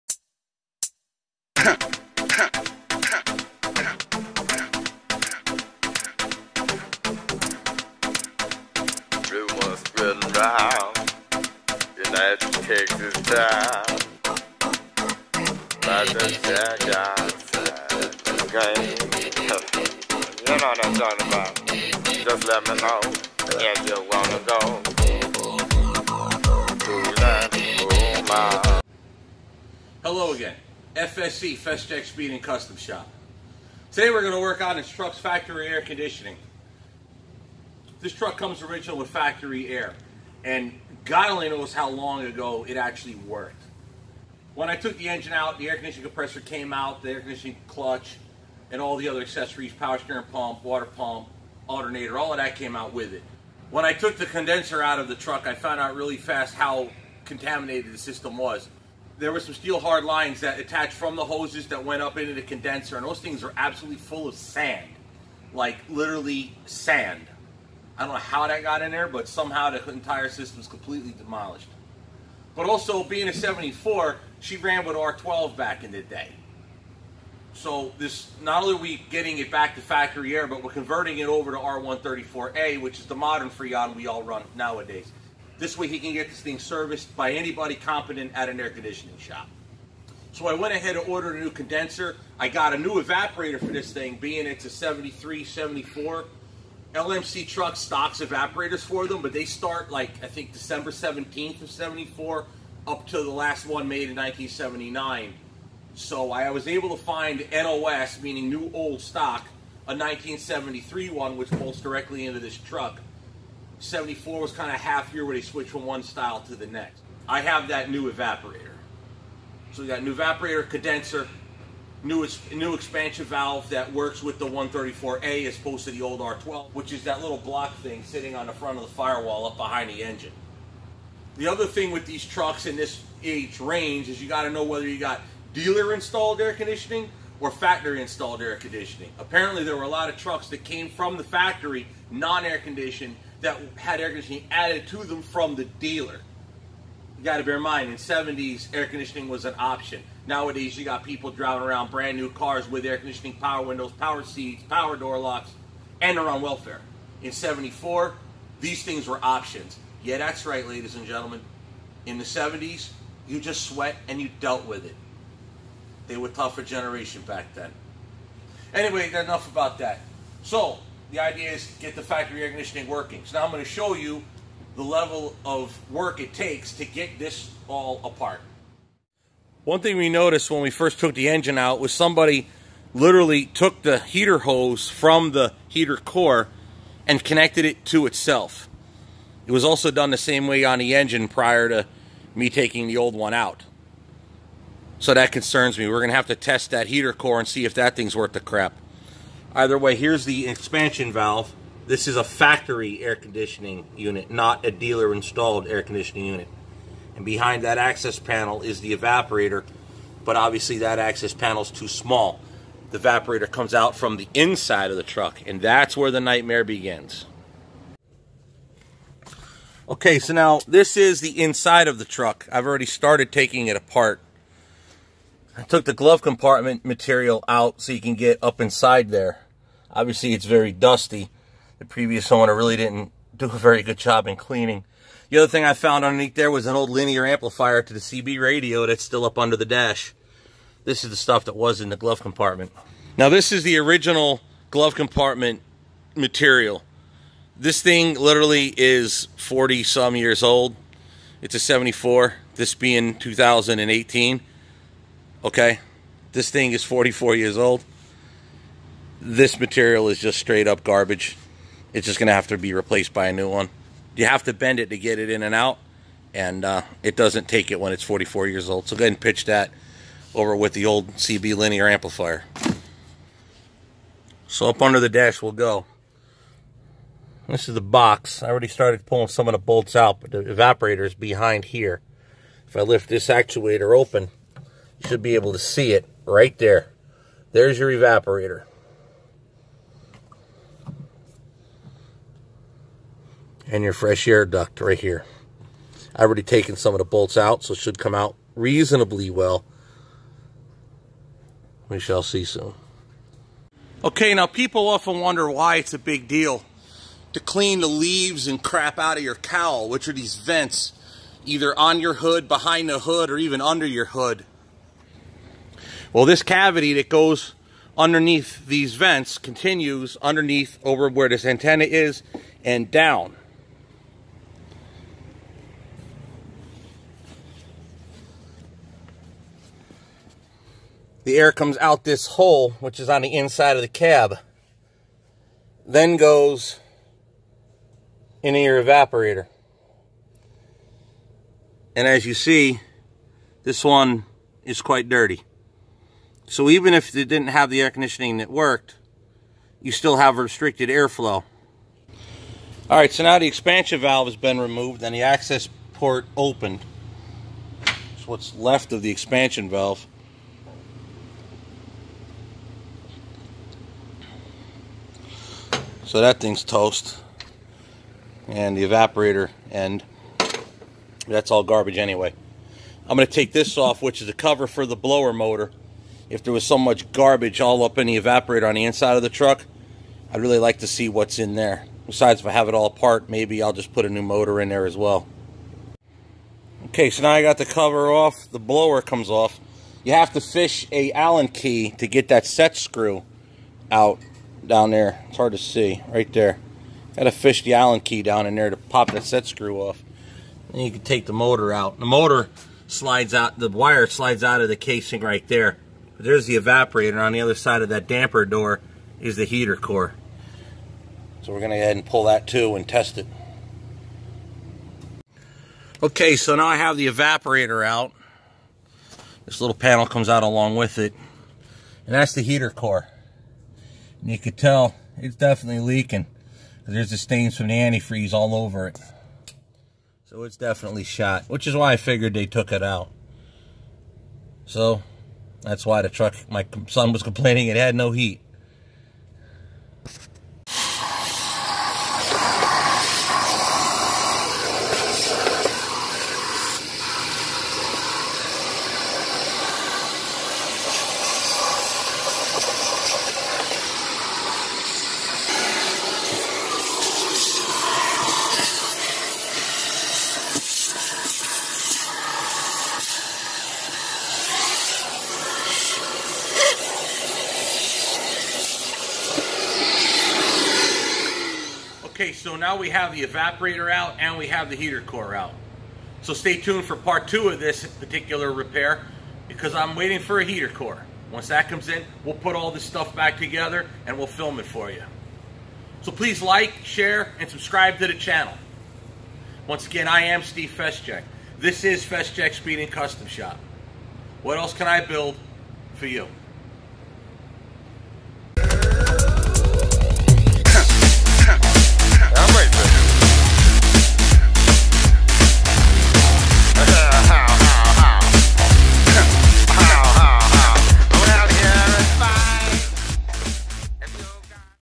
1974 Ford F-100 Factory A/C Evap & Heater Core Part 1
Removal of the Factory Air Conditioning Evaporator, Expansion Valve and Heater Core.  New evaporator  has arrived N.O.S. (New Old Stock).
Many parts coming from LMC Truck.

LMC Truck
LMCtruck.com